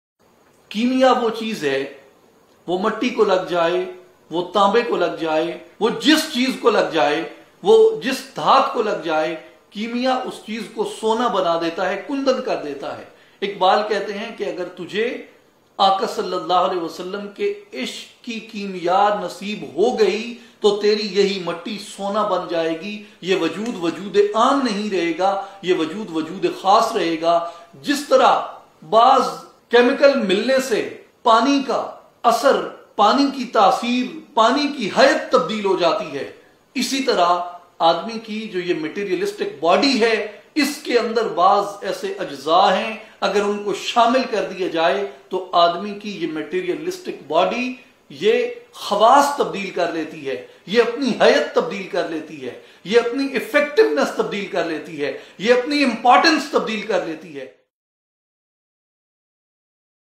Effects Of Love Of Prophet Muhammad (PBUH) on Existence.
[ Explained By: His Excellency Sahibzada Sultan Ahmad Ali Sb ]

عشقِ مصطفےٰﷺ کے وجود پر اثرات